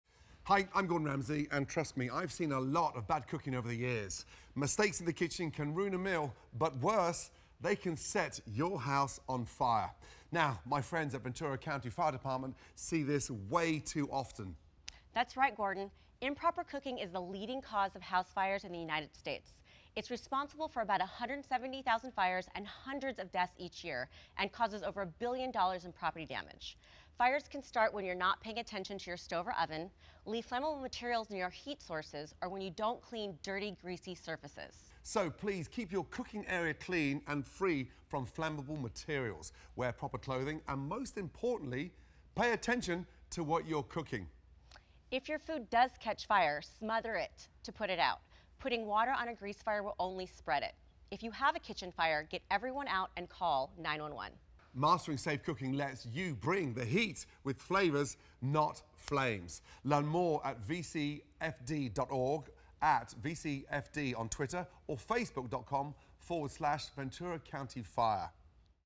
Master Chef Gordon Ramsey Kitchen Fire Safety PSA
Chef Gordon Ramsey and Captain Marisol Rodriguez describe the dangers of kitchen fires and how you can avoid them so so create great flavors not flames.